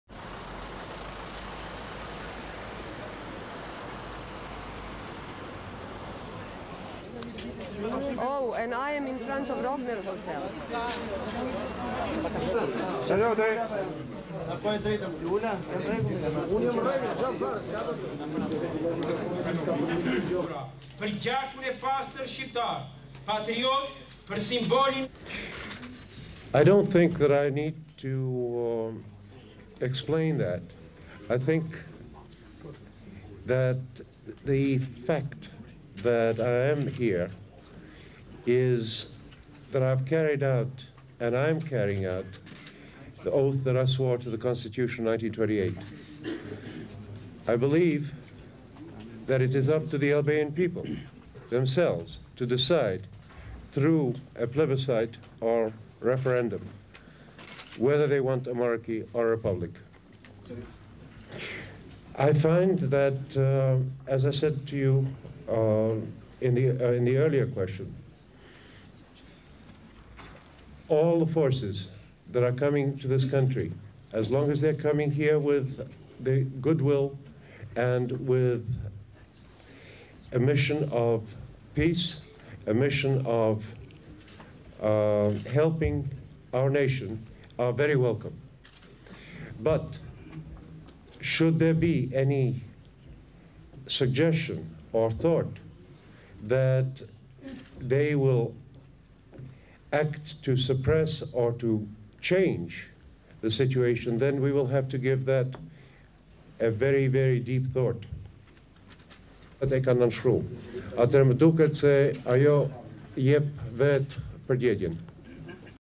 Albania - Exiled King Leka returns to his homeland
(12 Apr 1997) T/I 11:11:58

Albania's exiled king Leka returned to his homeland Saturday
receiving a raptuous welcome from a small crowd of well wishers
who had gathered at the capital's airport.
Later at a press conference, the self-styled King Leka II
claimed that he was the legitimate monarch of Albania.

SHOWS:

TIRANA, ALBANIA  12/4

Exterior Hotel Rogner;
MS people outside;
King Leka enters press conference;
SOT Leka (english) "I do not think that need to explain..I
think that the fact that I am here is that I am carrying out the
consititution of 1928..believe up to people themsleves to decide
whether want monarchy or republic..all forces that are coming to
country as long as with goodwill and with mission of peace are
welcome but should there be any suggestion or thought that they
will act to suppress or change the situation then we will have
to give deep thought."
WS press conference;

RUNS: 1.56